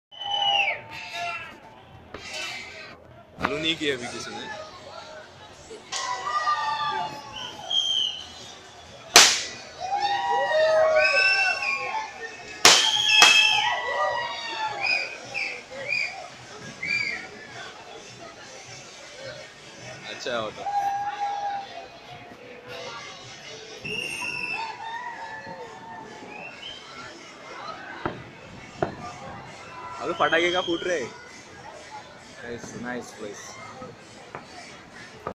New Years Celebrations @ KOREGAON PARK 2018
New Years Celebrations @ KOREGAON PARK 2018 at neight party in CLUBS with hot chicks around